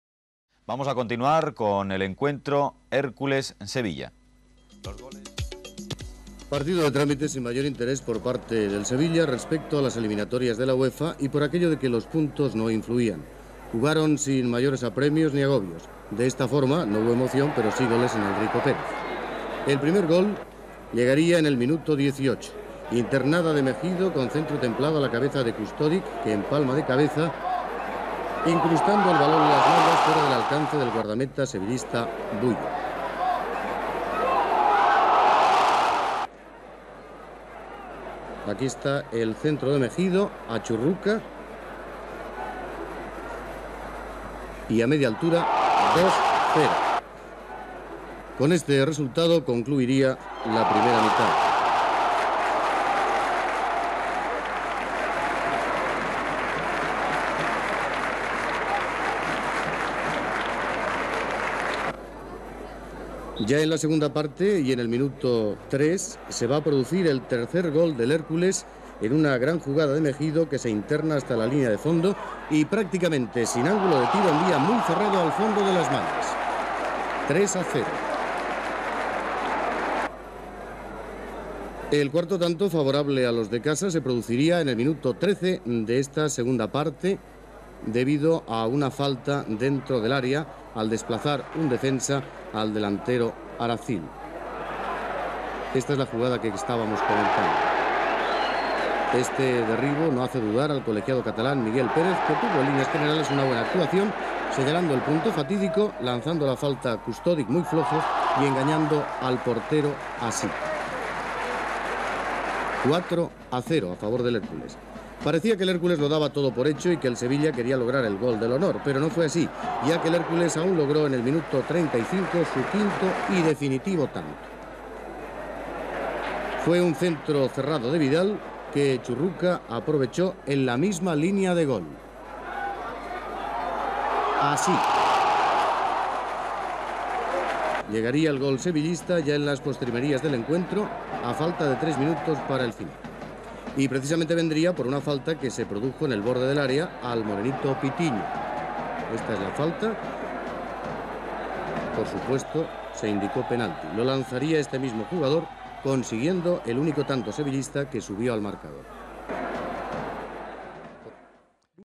1980-81 Hércules-Sevilla 5-1. Último partido de Kustudic
RTVE Conexión Vintage. Resumen Hércules-Sevilla / Temp. 1980-81 / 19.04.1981

HÉRCULES: Sala, Serrat, Baena, Cartagena, Carcelén, Juan, Aracil, Vidal, Megido, Kustudic, Churruca
SEVILLA: Buyo, Blanco, San José, Rivas, Alvarez, Pintinho, Juan Carlos (Juanito), Choya, Yiyi, Joaquín, Montero

Kustudic se despide del Hércules con dos goles. El entrenador Koldo Aguirre declara que la temporada que viene "O Kustudic o yo"...